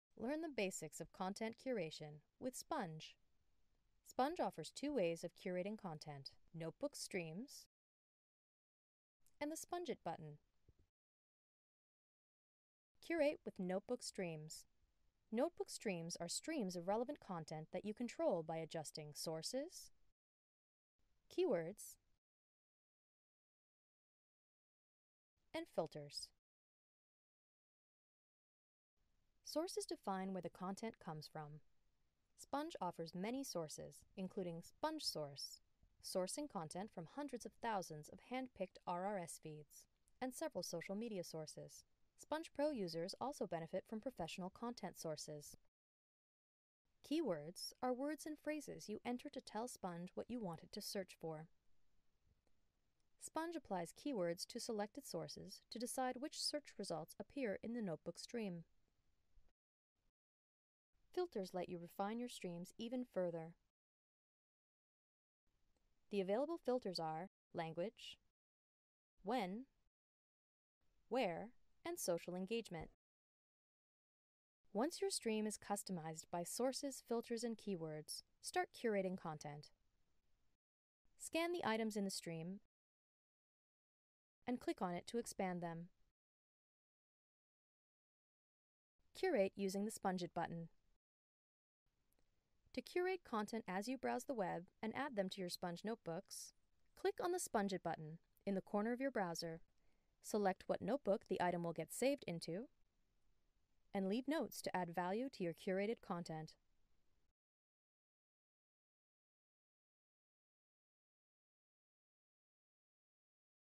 Knowledge Base Series: How to curate content with Spundge
Learn the basics of content curation with Spundge. Spundge offers two ways of curating content; Notebook Streams and the Spundge It button.